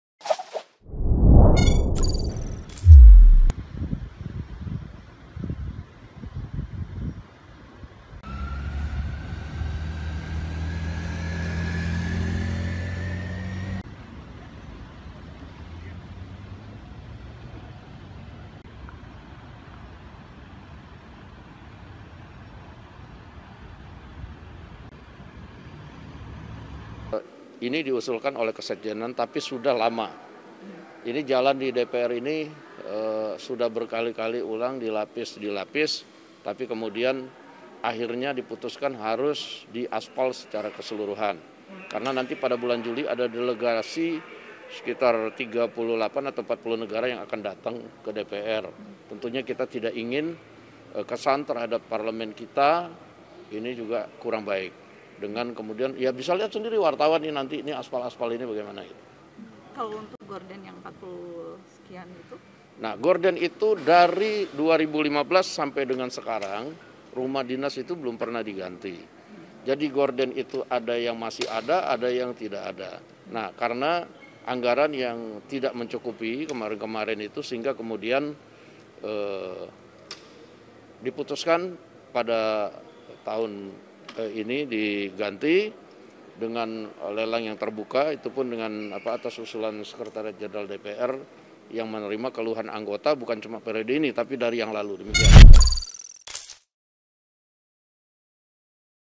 Ini Kata Pimpinan DPR soal Anggaran Puluhan Miliar Ganti Gorden-Aspal
Wakil Ketua DPR, Sufmi Dasco Ahmad menjelaskan anggaran puluhan miliar untuk mengganti gorden dan aspal di kompleks parlemen. DPR beralasan penggantian aspal untuk menyambut delegasi 40 negara bulan Juli mendatang. Kalau menurut kalian gimana guys?